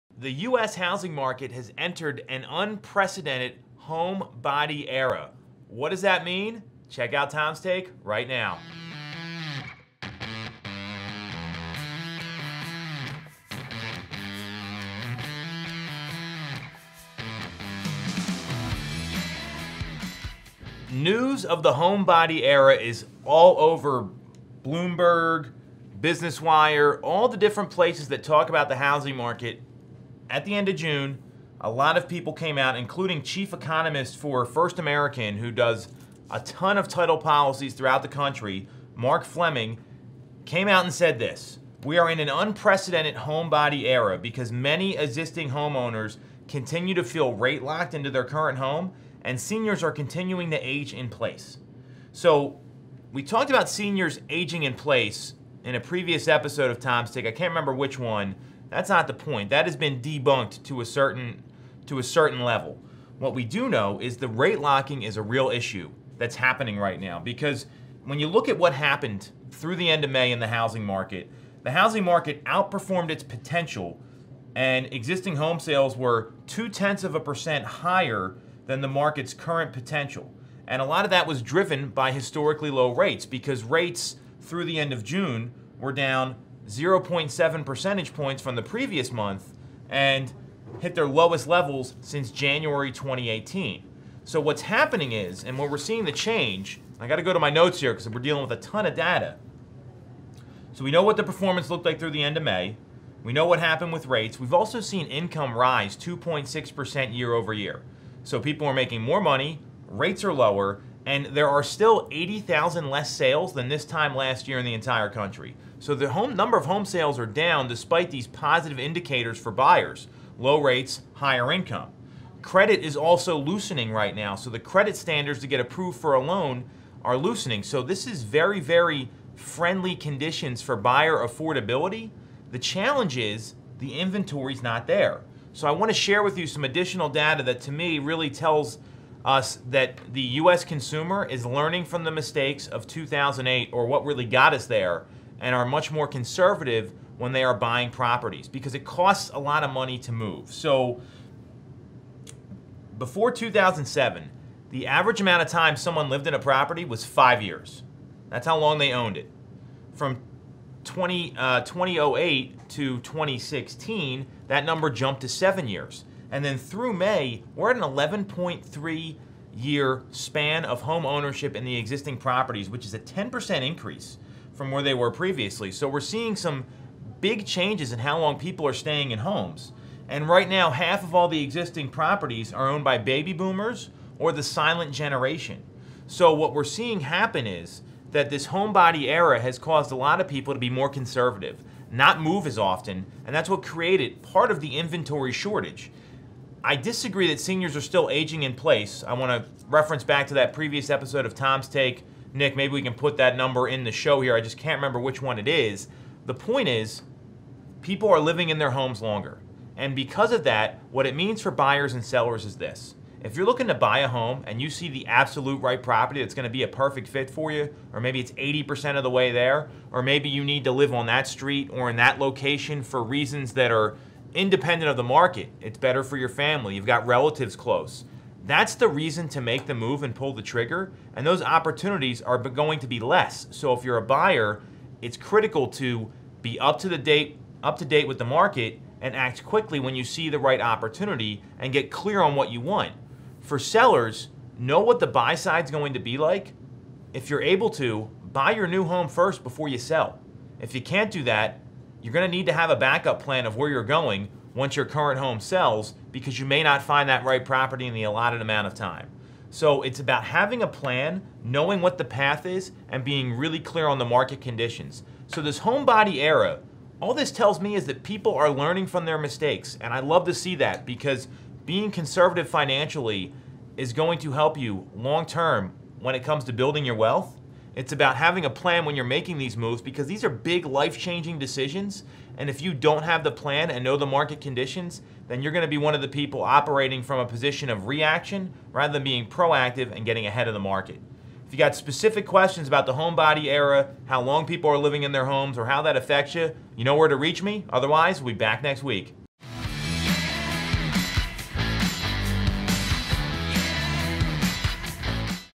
Tom's Take #123 - The Homebody Era
News of the homebody era is everywhere when researching real estate on the Internet.  People are staying in their homes longer.  Have consumers learned from the mistakes of the pre-market collapse in the early 2000's?  What does it mean for the real estate market?  Tune into episode #123 of Tom's Take now to get all the answers.